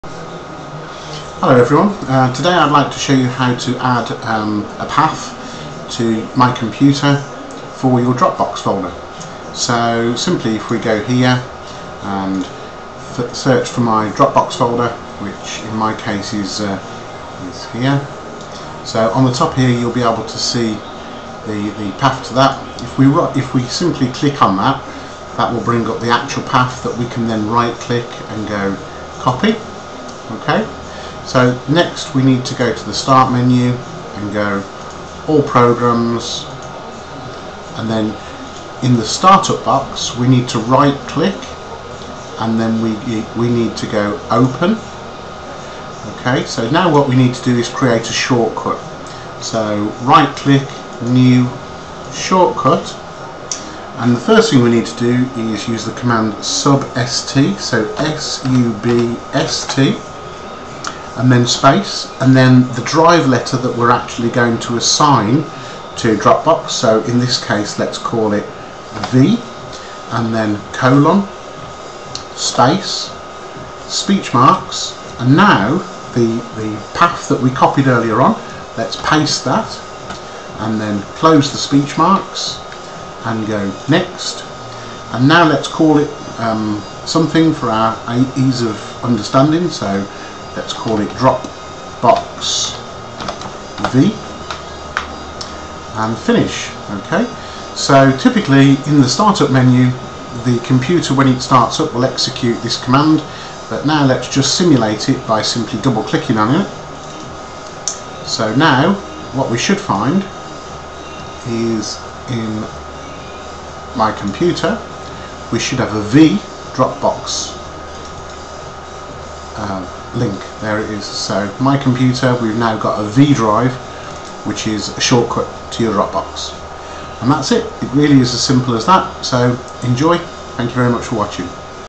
Mapping a drive letter to dropbox folder
This short video shows you how to map your dropbox folder with a drive letter on your pc so that you can easily access the folder from your pc.